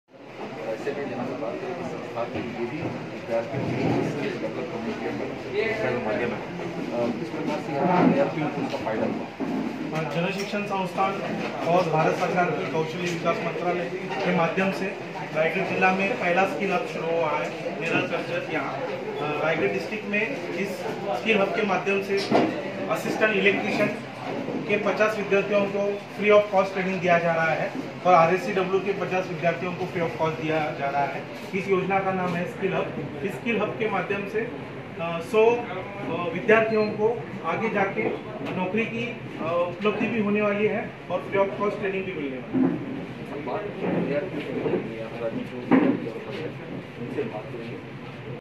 WJan Shikshan Sansthan Raigad Attend the Live Programe of Pariksha Pe Charcha
Jan Shikshan Sansthan Raigad Attend the Live Programe of Pariksha Pe Charcha Interacted by The Honorable Prime Minister Shri Naredra Modi Ji With The beneficiaries of JSS All Over The Country on 1st April 2022 We Attend The Program With Skill Hub beneficiaries of Assistant Electrician & Field Engineer RACW Job Role Under Skill Hub Initiative Scheme at Jan Shikshan Sansthan Raigad Maharashtra Neral Karjat Centre in Presence of Dr. Nitin B Gandhi (Chairman JSS Raigad), Director Mr Vijay Kokane, Mr Navin Bhopi (Placement Coordinator),Mr Ganesh Bhopi (Assistant Electrician Trainer) Mr. Vijay Ranadive (Field Engineer RACW Trainer), Mr Avinash Dyare And The Staff of Jan Shikshan Sansthan Raigad Mrs. Kalpana Mhatre (Assistant Program Officer), Mr Omkar Borkar & Himanshu Bhalkar